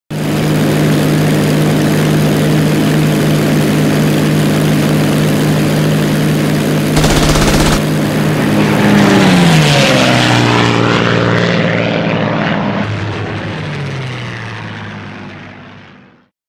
Spitfire Element 3D V2 Test
Thank you to Anders Lejczak (bazze) for creating this spitfire which I got on TF3DM site. I used CC forced motion blur on the propeller. But it takes forever to render. I have tried the propeller blur kit but it look fake unless the plane is a fair distance away. I had to split the propeller then used it on group 2 in Element 3D.